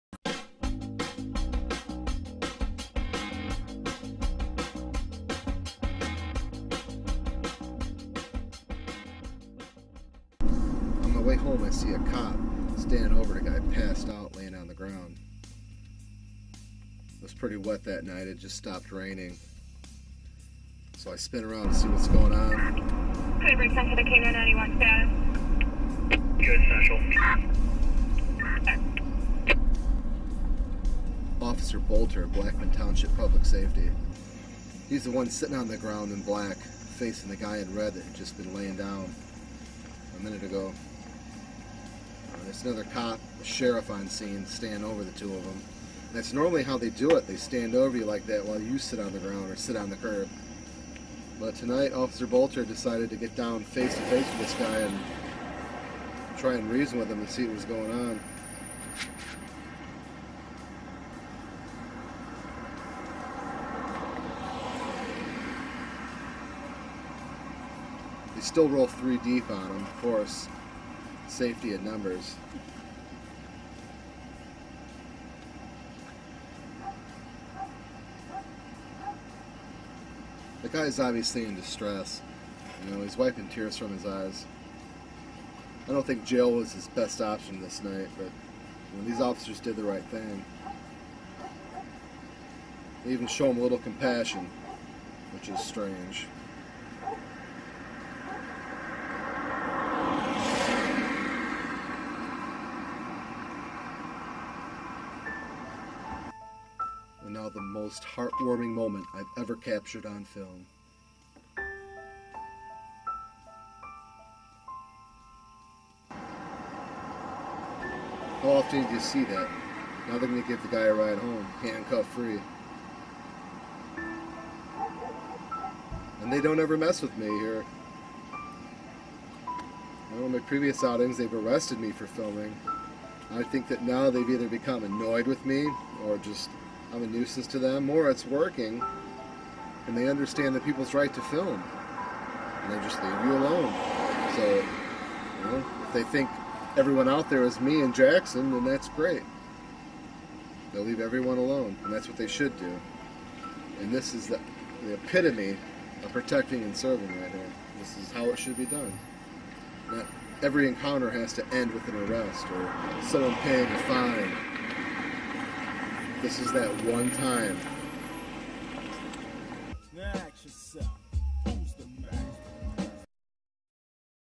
Policja robi dobry uczynek...
Policja. facet zemdlał, na parkingu naprzeciw lokalnym barze, trochę życzliwości.